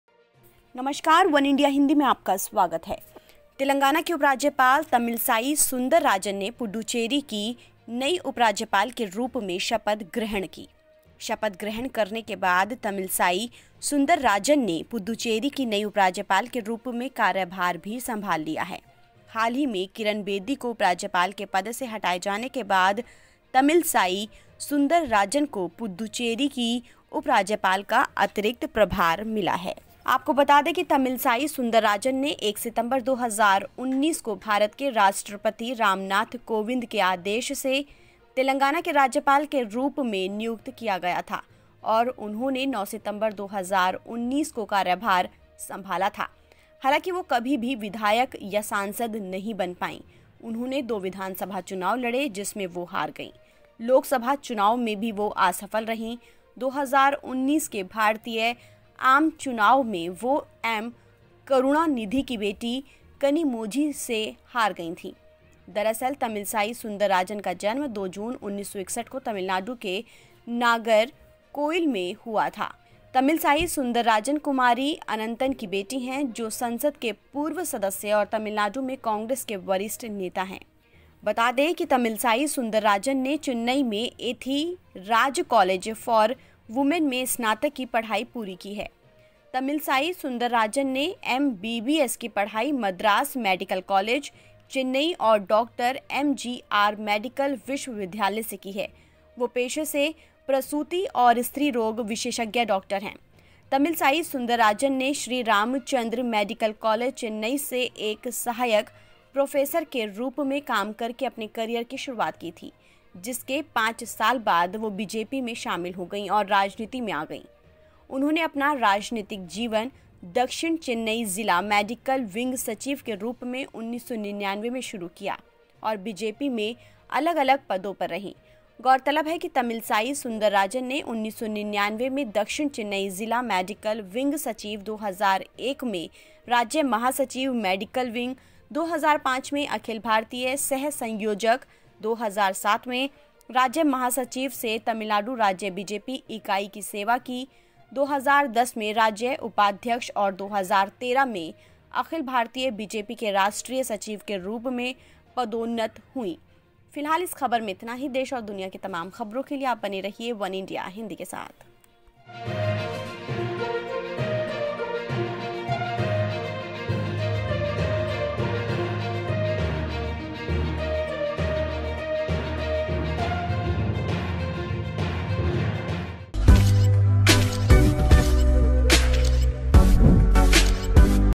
Puducherry की Lt. Governor बनीं Tamilisai Soundararajan, जानिए कौन हैं ये | वनइंडिया हिंदी
Telangana Governor Tamilisai Soundararajan was sworn in as the new Lieutenant Governor of Puducherry. After swearing in, Tamilasai Sundararajan has also taken over as the new Lt. Governor of Puducherry. Recently after Kiran Bedi was removed from the post of Lieutenant Governor, Tamilisai Soundararajan has got additional charge of Lieutenant Governor of Puducherry.

तेलंगाना की राज्यपाल तमिलसाई सुंदरराजन ने पुडुचेरी की नई उपराज्यपाल के रूप में शपथ ग्रहण की। शपथ ग्रहण करने के बाद तमिलसाई सुंदरराजन ने पुडुचेरी की नई उपराज्यपाल के रूप में कार्यभार भी संभाल लिया है। हाल ही में किरण बेदी को उपराज्यपाल के पद से हटाए जाने के बाद तमिलसाई सुंदरराजन को पुडुचेरी की उपराज्यपाल का अतिरिक्त प्रभार मिला है।